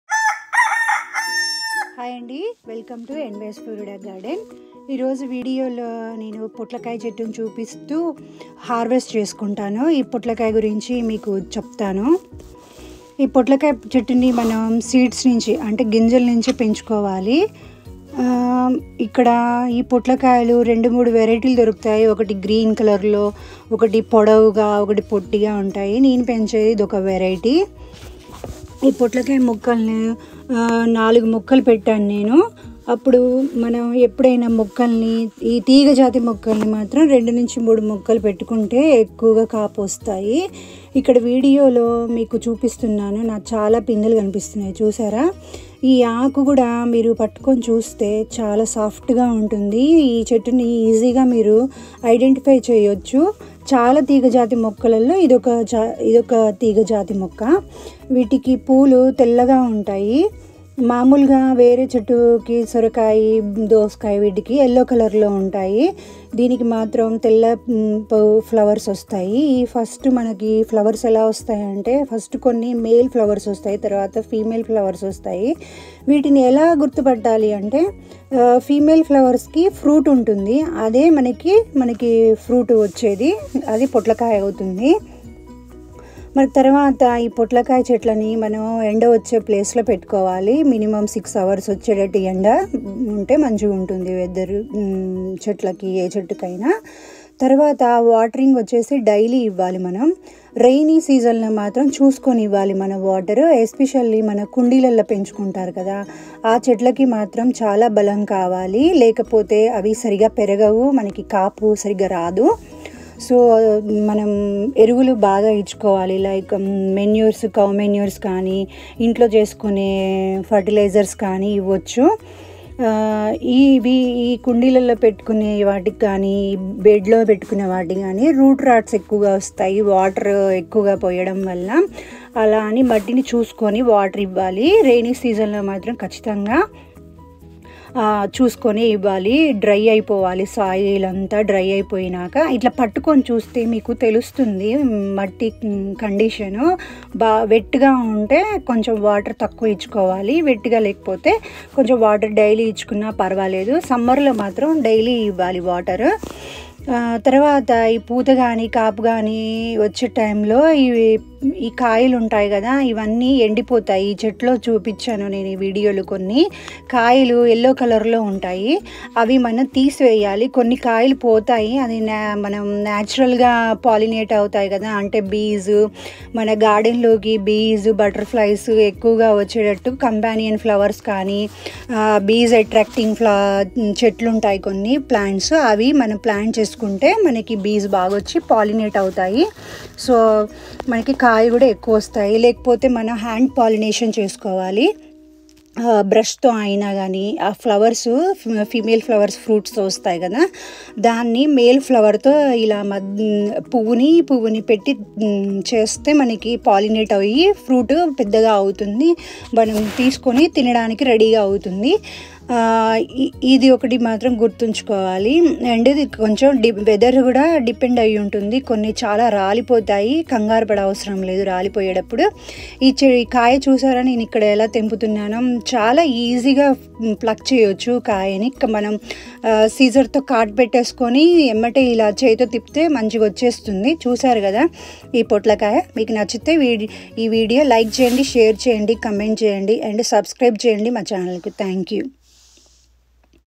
Zone 8b-13|| showing my Snake Gourd#Potlakaya plants and harvest from the garden
Hi andi, Welcome to "NY's Florida Garden" . Today in this video, showing my snake gourd plants and harvesting snake gourds from my backyard garden.
Please watch completed video and enjoy the harvest and small tips.
"NY's Florida Garden".